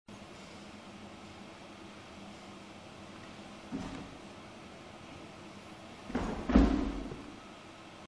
Handstandvarianten (13)
Im Gerätturnen sind verschiedene Turnelemente in Schwungübungen, statische Übungen und Übungen ohne Schwung einzuteilen. Diese Kategorisierung erfolgt nach strukturellen Aspekten, welche aufgezeigt werden.